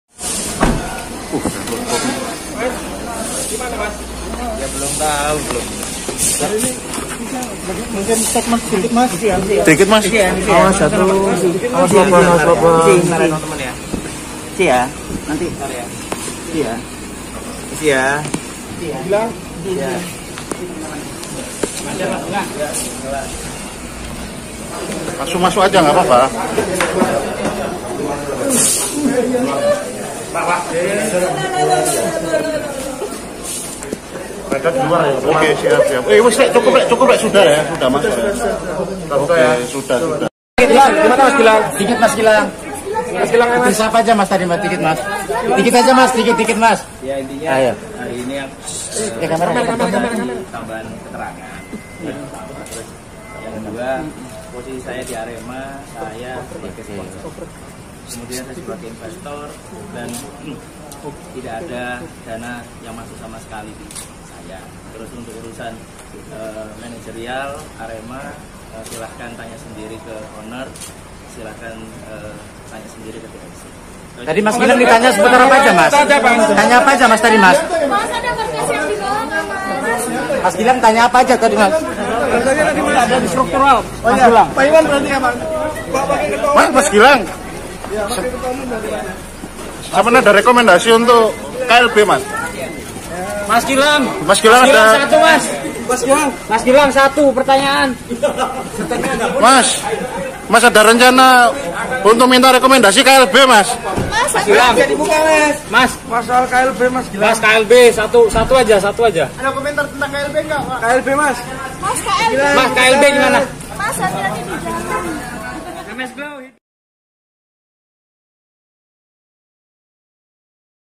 CURHAT BOS JURAGAN99 USAI DIPERIKSA POLDA JATIM TERKAIT TRAGEDI KANJURUHAN
CURHAT BOS JURAGAN99
USAI DIPERIKSA POLDA JATIM TERKAIT TRAGEDI KANJURUHAN

REPORTER: LUHUR PAMBUDI | YUL

TRIBUNJATIM.COM, SURABAYA - Presiden Arema FC, Gilang Widya Pramana terbilang irit bicara usai diperiksa sebagai saksi atas kasus Tragedi Kanjuruhan Malang, yang menewaskan 135 orang, di Mapolda Jatim, Kamis (27/10/2022).

Kepada awak media yang menunggunya sejak pagi hingga malam hari, Juragan 99, julukannya itu, hanya menyampaikan informasi perihal pemeriksaan yang baru dijalaninya tadi, seperlunya.

Bahkan, seusai menyampaikan penjelasan kepada awak media.

Gilang berusaha memungkasi sesi wawancara tersebut, secepat mungkin.

Saking ingin segera menyelesaikan sesi wawancara tersebut.

Gilang bahkan harus melangkah melompati kolam ikan hias yang terdapat di dekat pintu keluar Gedung Ditreskrimum Mapolda Jatim tersebut.

Presiden Arema FC Gilang Widya Pramana mengatakan, dirinya hanya memberikan keterangan tambahan dalam agenda pemeriksaan tersebut.

Posisi dan jabatan dirinya di dalam klub berjuluk 'Singo Edan' tersebut, sebatas sebagai sponsor.

Itupun, Gilang mengklaim, sebagai investor, dirinya belum memperoleh keuntungan sama sekali.

"Ya intinya saya pada hari ini dimintai tambahan keterangan. Yang Kedua, posisi saya di Arema saya sebagai sponsor, kemudian saya sebagai investor dan tidak ada dana yang masuk sama sekali ke saya," ujarnya di depan Gedung Ditreskrimum Mapolda Jatim, Kamis (27/10/2022).

Mengenai teknisi manajerial, Gilang menyerahkan semua informasi tersebut kepada pihak owner Arema FC.

"Untuk urusan manajerial Arema, silakan tanya sendiri ke owner. Silakan tanya sendiri," pungkasnya.